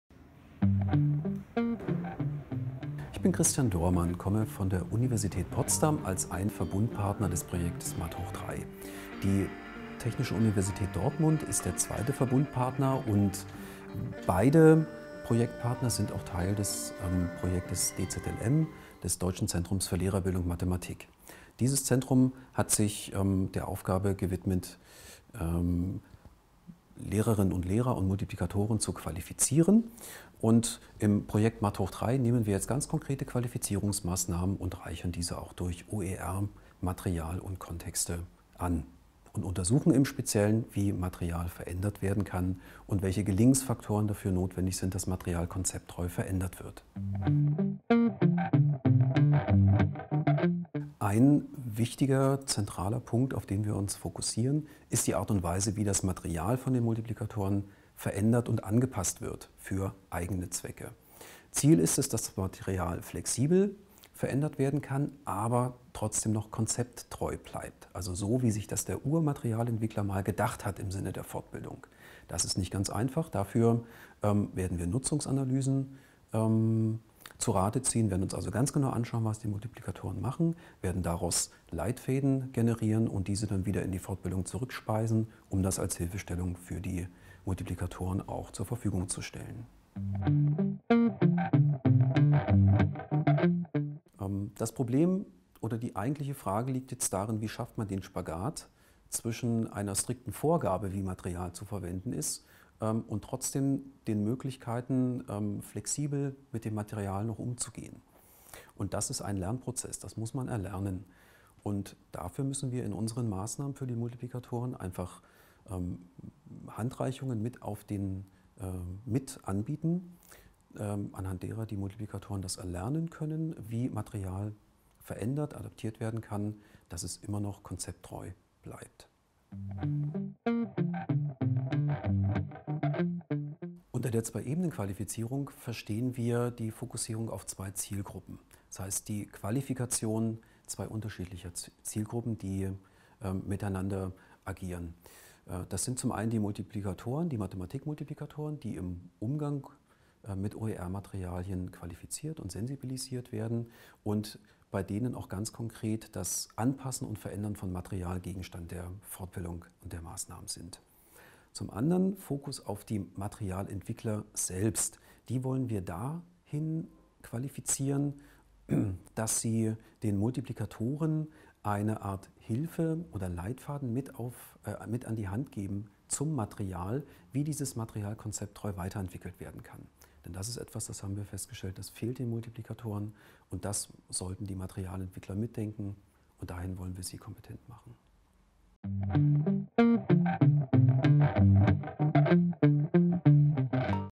OERinfo – Christian Dohrmann (Math^3) im Interview
OER-Projekte im Porträt – Folge 2: Christian Dohrmann im Interview

Im Projekt Math^3 werden konkrete Qualifizierungsmaßnahmen zu OER für Lehrende der Mathematik und für Multiplikator*innen angeboten. Dabei liegt der Schwerpunkt, wie Projektmitarbeiter Christian Dohrmann erklärt, auf der Frage, wie das Material von den Multiplikator*innen verändert und angepasst werden kann.

Der Name des Urhebers soll bei einer Weiterverwendung wie folgt genannt werden: J&K – Jöran und Konsorten für die Informationsstelle OER.